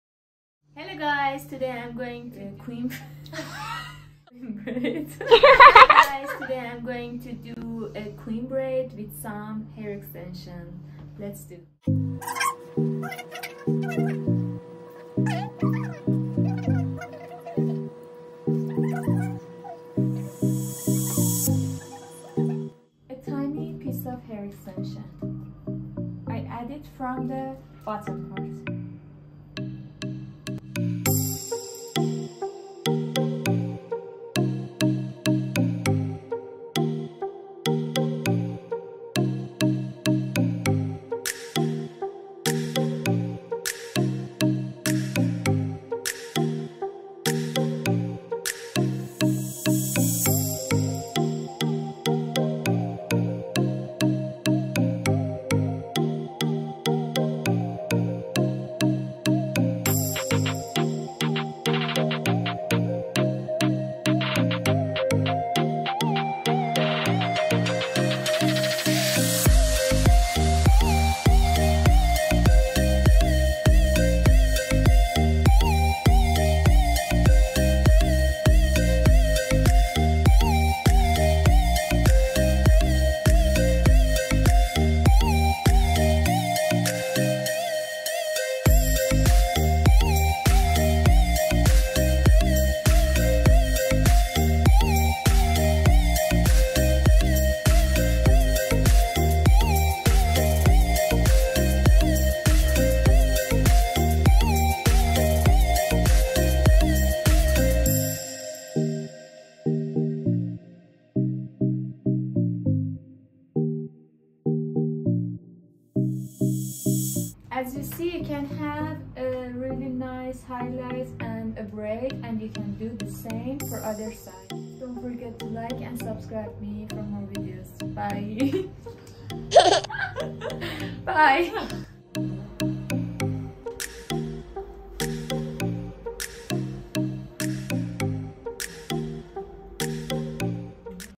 Queen braid
Have some highlights on your hair by adding hair extensions and braid